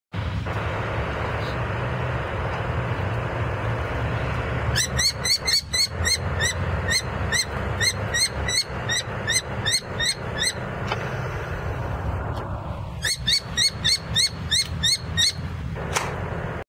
Kestrels after ringing + sharp voice Hrodna 06.06.2011 by Dz.Vincheuski.mov
3 juveniles of Kestrel after ringing on the nest on Catholic church in Hrodna, Western Belarus. Fourth juv. escaping behind the statue and was not ringed.
Тры птушаня сокала-пустальгі былі акальцаваныя на гняздзе за статуяй Св.Алены на былым бернардынскім касцёле, Гродна, Беларусь. Чацьвёрты схаваўся за статуяй і застаўся без кальца. Адзін з малых вельмі гучна крычыць.Відэа зроблена Дзьмітрыям Вінчэўскім для